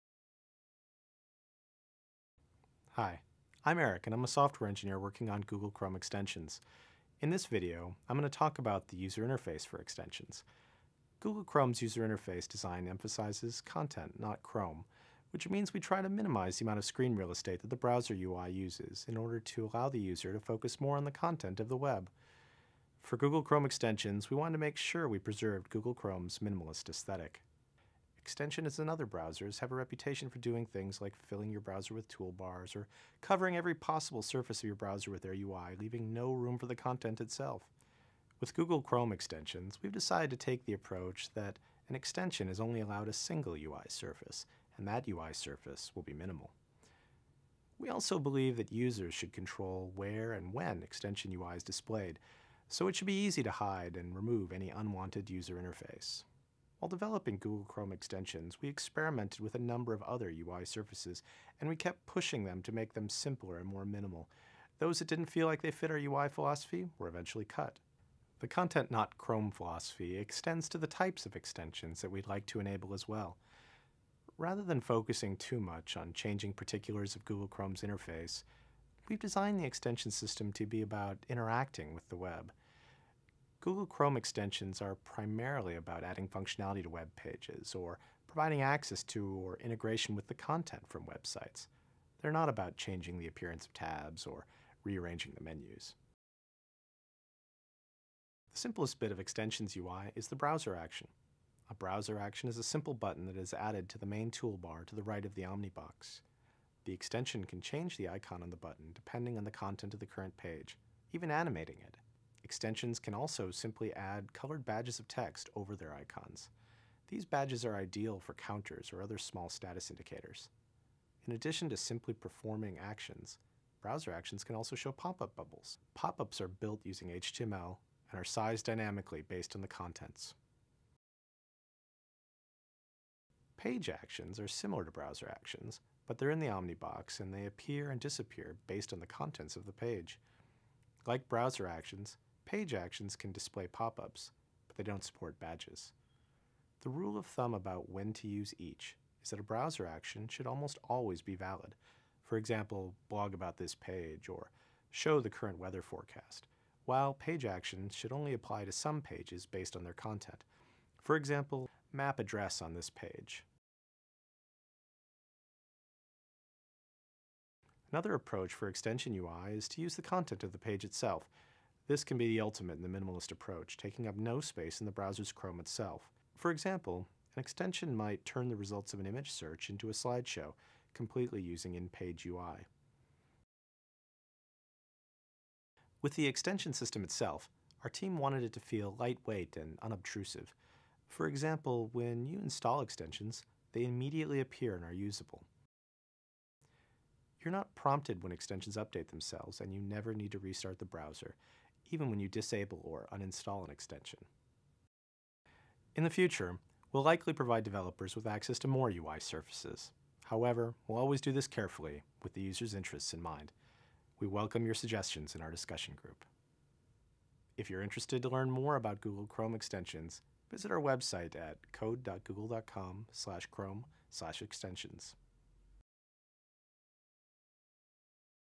Google Chrome Extensions: UI Design
Erik Kay, an engineer at Google, provides more information about the UI of Google Chrome's extension system.